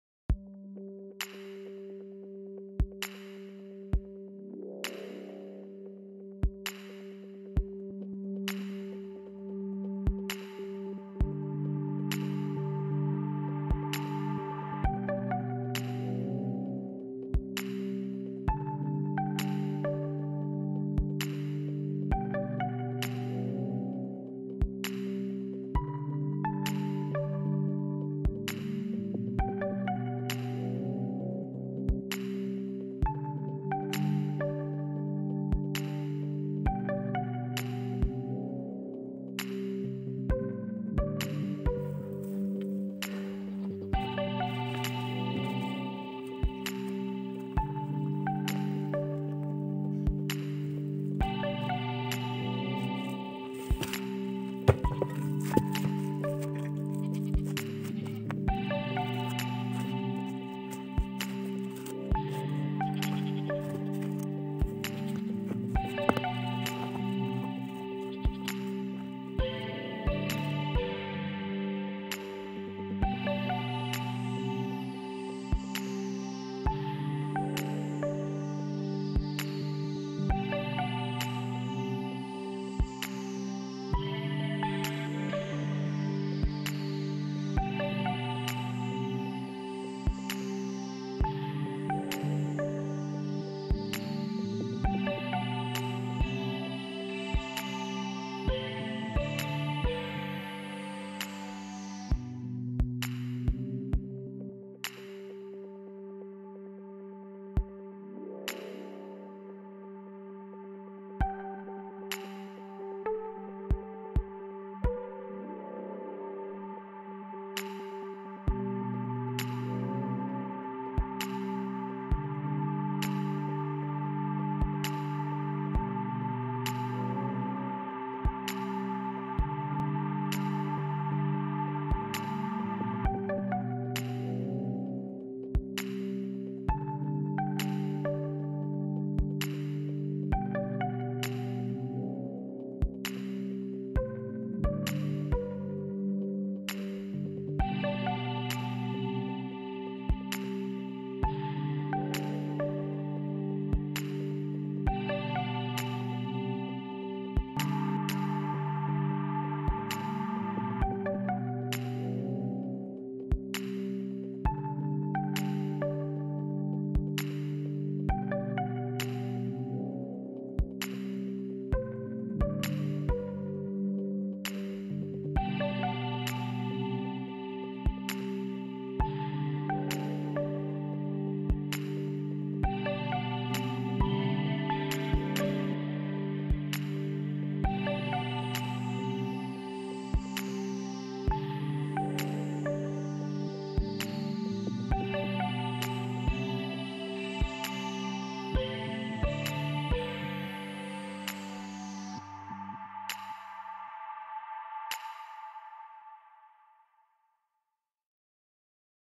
Helping Our Neighbors Build Fieldstone Wall | How We Build Our Fields Walls in Hunza Valley Pakistan
Helping Our Neighbors  Build Fieldstone Wall in | Salamat. Fields in Hunza Valley Pakistan are supported by small stone walls. Often they fall due to water over spilling and weathering.

In this video I helped Our Neighbors  to build fieldstone wall with different size stones.